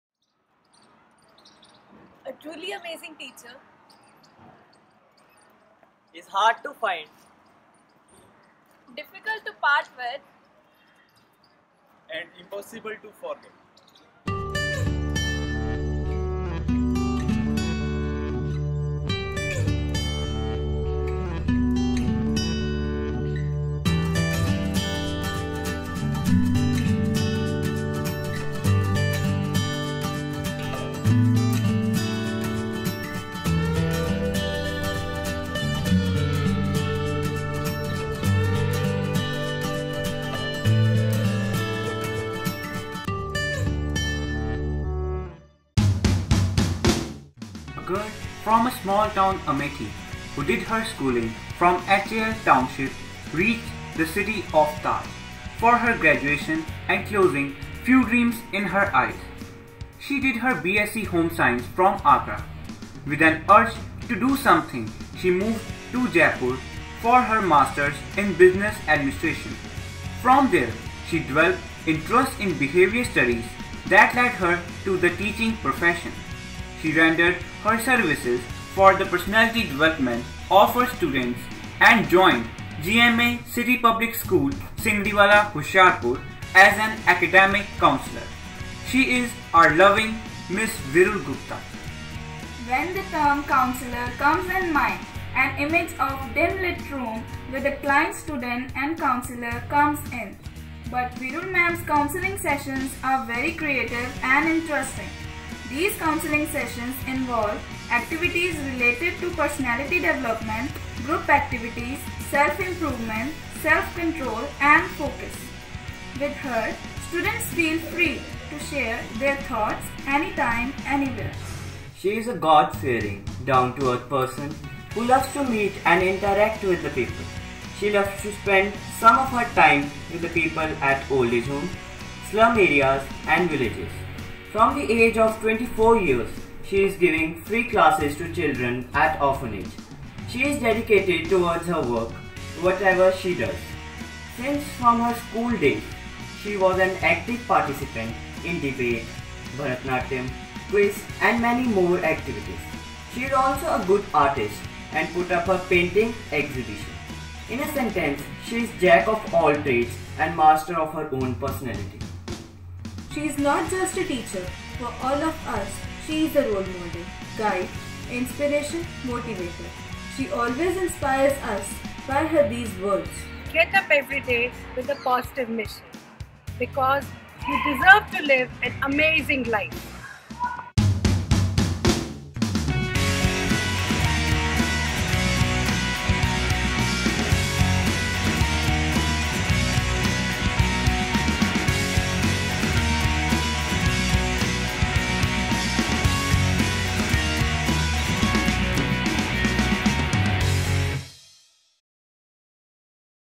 LPU Transforming Education Awards Entry | Ms Veerul Gupta | GMACPS | QUADD
This video is submission for LPU Transforming Education Awards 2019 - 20 by following students for GMA City Public School, Singriwala Campus:
Megha, Ashutosh, Gurleen, Eknoor and Gurwinder